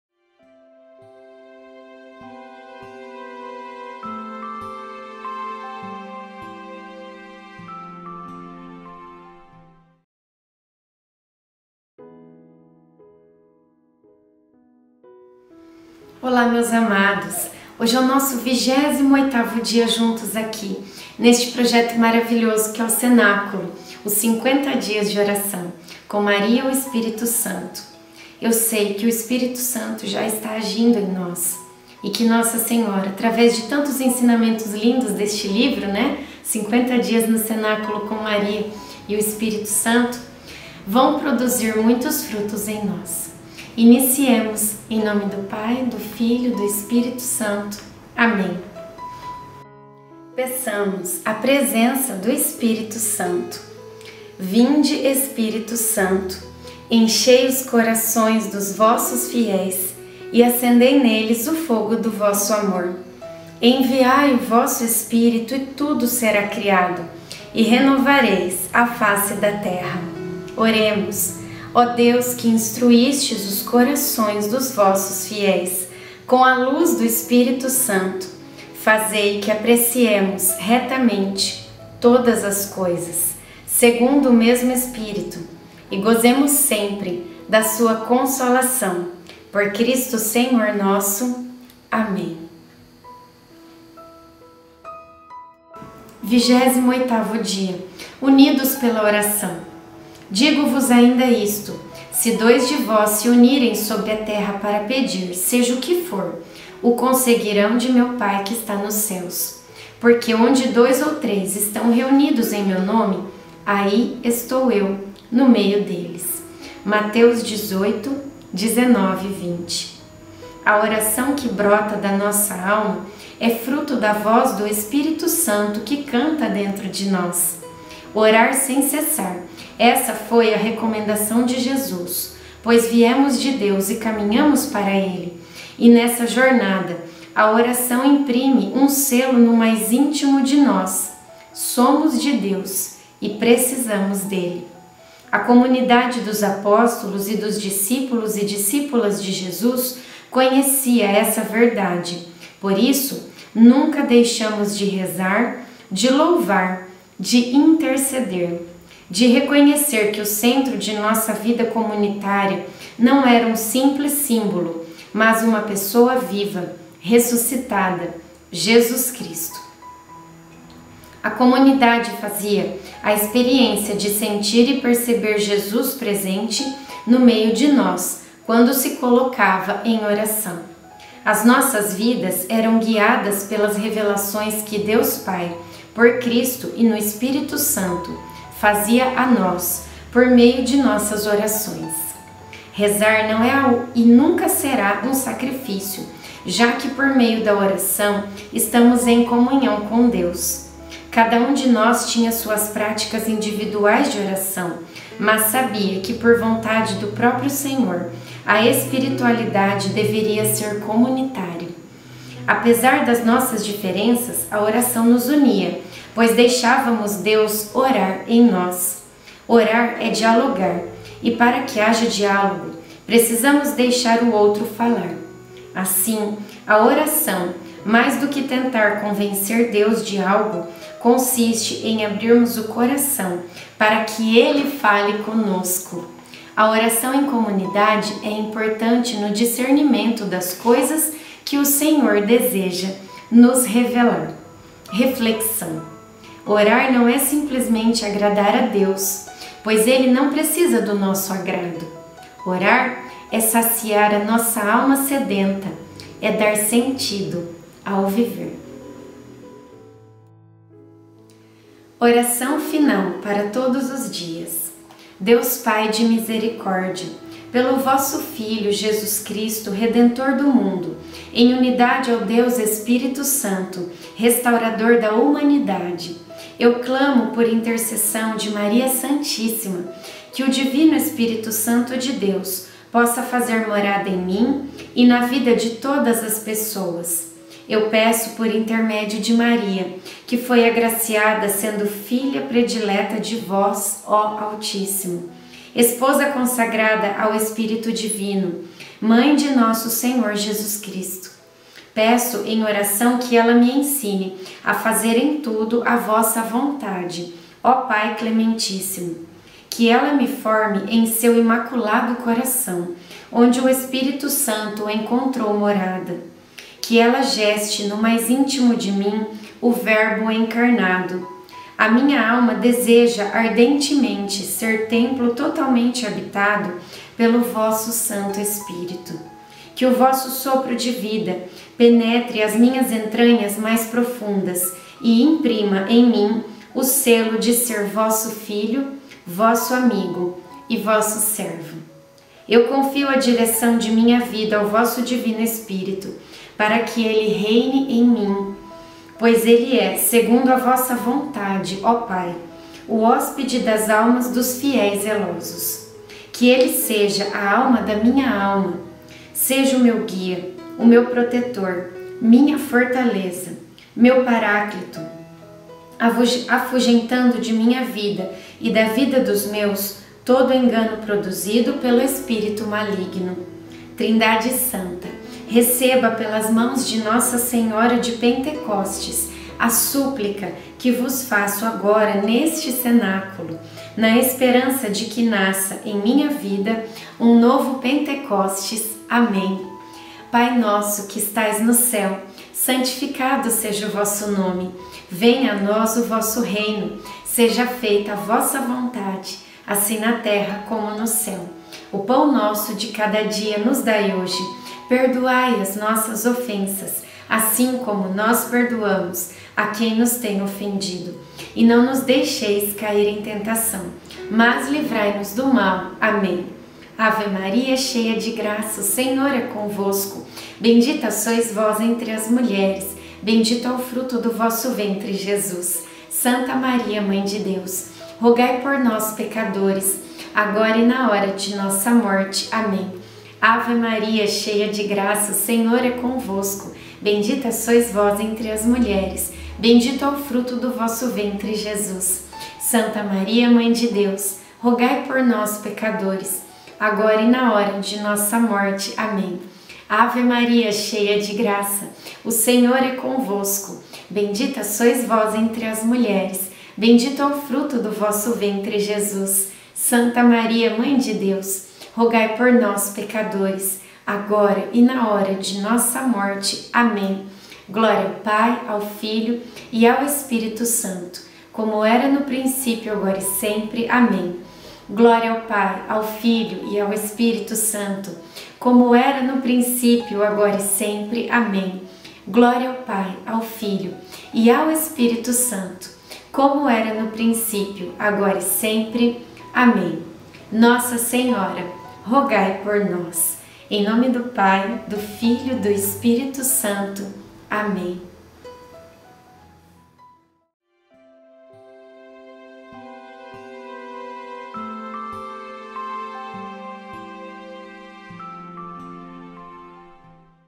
50 DIAS NO CENÁCULO COM MARIA E O ESPÍRITO SANTO 28
28o Dia — Unidos pela oração
“Digo-vos ainda isto: se dois de vós se unirem sobre a Terra para pedir, seja o
que for, o conseguirão de meu Pai que está nos Céus. Porque onde dois ou três
estão reunidos em meu nome, aí estou eu no meio deles.”
(Mateus 18,19-20)
oração que brota na nossa alma é fruto da voz do Espírito Santo que canta
dentro de nós. Orar sem cessar, essa foi a recomendação de Jesus, pois
viemos de Deus e caminhamos para Ele, e, nessa jornada, a oração imprime
um selo no mais íntimo de nós: “Somos de Deus e precisamos dele”. A
comunidade dos apóstolos e dos discípulos e discípulas de Jesus conhecia
essa verdade. Por isso, nunca deixamos de rezar, de louvar, de interceder; de
reconhecer que o centro da nossa vida comunitária não era um simples
símbolo, mas uma pessoa viva, ressuscitada, Jesus Cristo. A comunidade
fazia a experiência de sentir e perceber Jesus presente no meio de nós quando
se colocava em oração. As nossas vidas eram guiadas pelas revelações que
Deus Pai, por Cristo e no Espírito Santo, fazia a nós por meio de nossas
orações. Rezar não é e nunca será um sacrifício, já que por meio da oração,
estamos em comunhão com Deus. Cada um de nós tinha suas práticas
individuais de oração, mas sabia que, por vontade do próprio Senhor, a
espiritualidade deveria ser comunitária. Apesar das nossas diferenças, a
oração nos unia, pois deixávamos Deus orar em nós. Orar é dialogar, e, para
que haja diálogo, precisamos deixar o outro falar. Assim, a oração — mais do
que tentar convencer Deus de algo — consiste em abrirmos o coração para
que Ele fale conosco.
A oração em comunidade é importante no discernimento das coisas que o
Senhor deseja nos revelar.
Orar não é simplesmente agradar a Deus, pois Ele não precisa do nosso
agrado. Orar é saciar a nossa alma sedenta, é dar sentido ao viver.

Oração

Deus Pai de Misericórdia; pelo vosso Filho, Jesus Cristo, Redentor do
mundo; em unidade ao Deus Espírito Santo, restaurador da
humanidade, eu clamo por intercessão de Maria Santíssima, que o Divino
Espírito Santo de Deus possa fazer morada em mim e na vida de todas as
pessoas. Eu peço por intermédio de Maria, que foi agraciada sendo filha
predileta de vós, ó Altíssimo; esposa consagrada ao Espírito Divino; mãe de
nosso Senhor Jesus Cristo. Peço em oração que ela me ensine a fazer em tudo
a vossa vontade, ó Pai Clementíssimo; que ela me forme em seu Imaculado
Coração onde o Espírito Santo encontrou morada; que ela geste, no mais
íntimo de mim, o Verbo Encarnado. A minha alma, deseja ardentemente ser
templo totalmente habitado pelo vosso Santo Espírito. Que o vosso sopro de
vida, penetre as minhas entranhas mais profundas e imprima em mim o selo
de ser vosso filho (a), vosso amigo (a), vosso servo (a). Eu confio a direção
de minha vida ao vosso Divino Espírito, para que Ele reine em mim, pois Ele
é, segundo a vossa vontade ó Pai, o hospede das almas dos fiéis zelosos. Que
Ele seja a alma da minha alma; seja o meu guia; o meu protetor; minha
fortaleza; meu Paráclito; afugentando de minha vida e da vida dos meus, todo
engano produzido pelo espírito maligno. Trindade Santa, receba pelas mãos
de Nossa Senhora de Pentecostes a súplica que vos faço agora neste
Cenáculo, na esperança de que nasça em minha vida um novo Pentecostes.
Amém!

doações espontâneas
Deus abençoe!